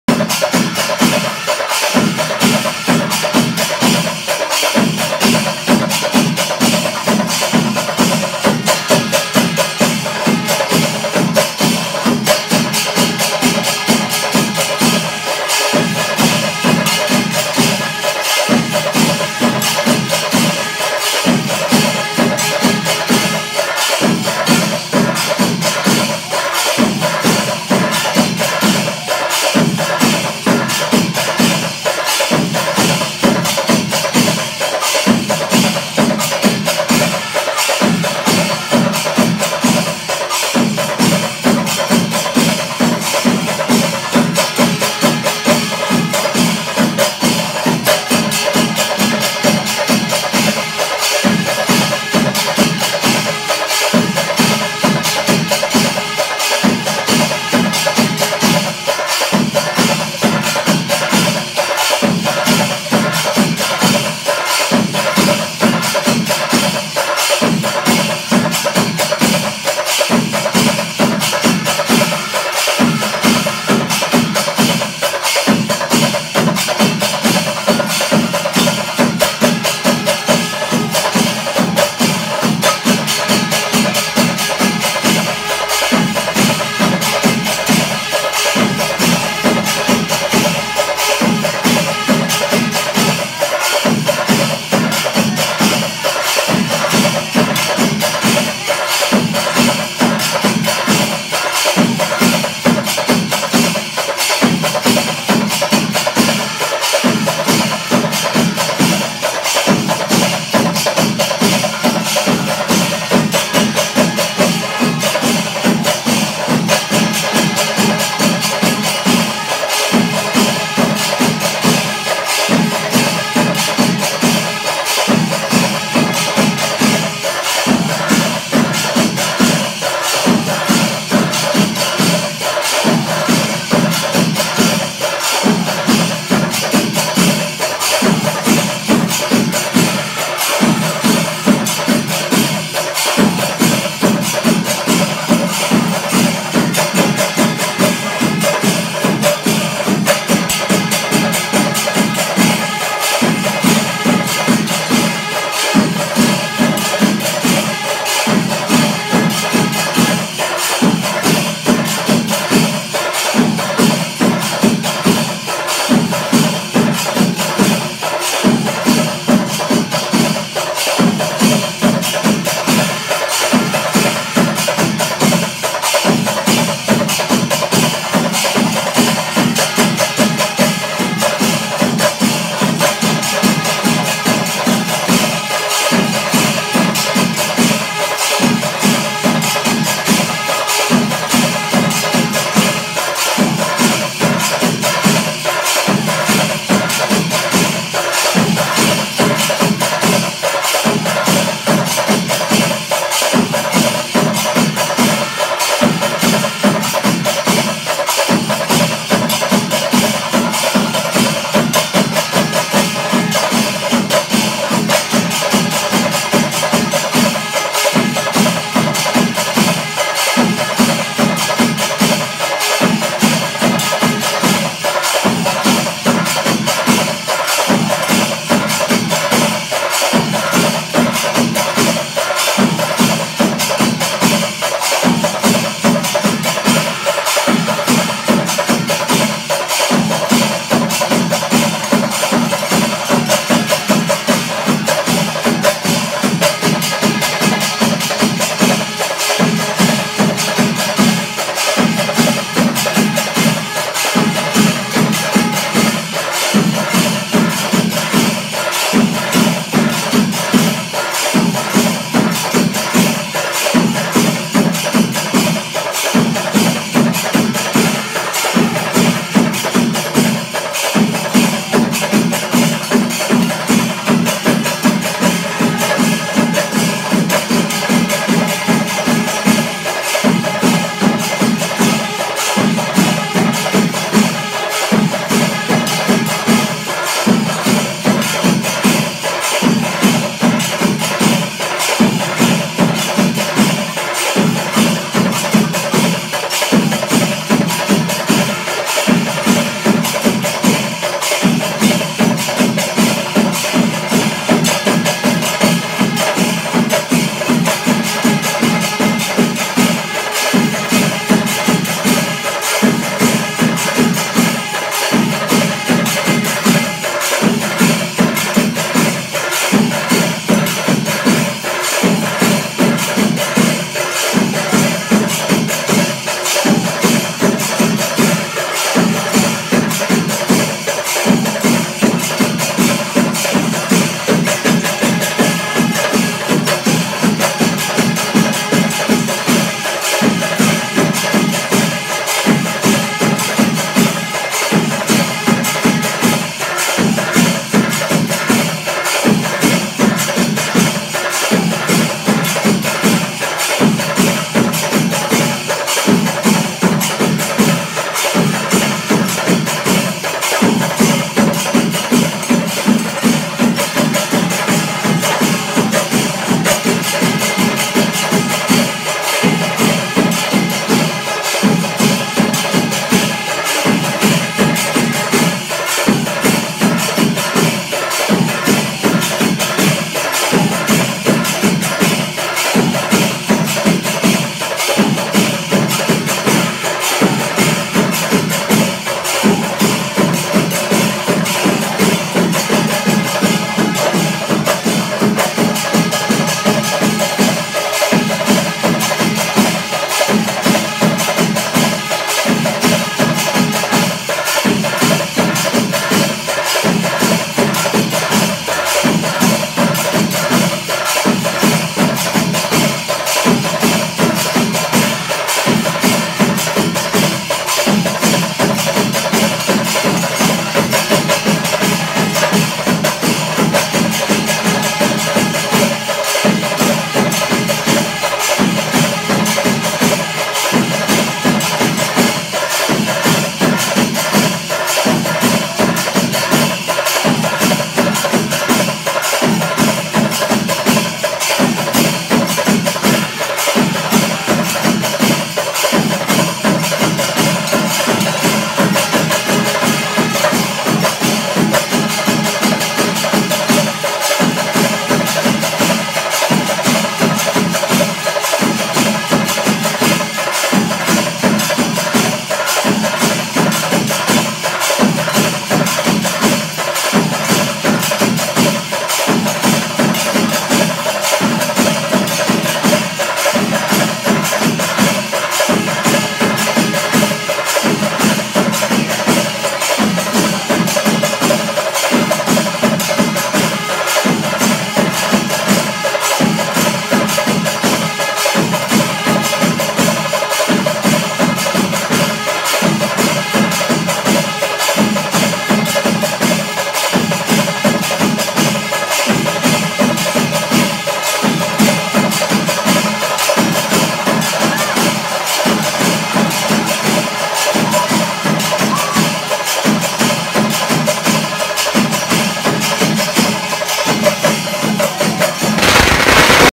Padmasree Peruvanam Kuttan Maarar Performing Kuttanellur panchari melam - 5th kaalam kalaasam
Kuttanellur Panchari Melam is ever famous by its traditional values. Many legendary percussion artists performed the famous Kuttanellur panchari which is even accompanied by the full moon in the sky.Padmasree peruvanam Kuttan Maarar leads the famous kuttanellur panchari melam for the year 2018.Its one of the longest pancharimelam which strictly adhere to the traditional melam rules.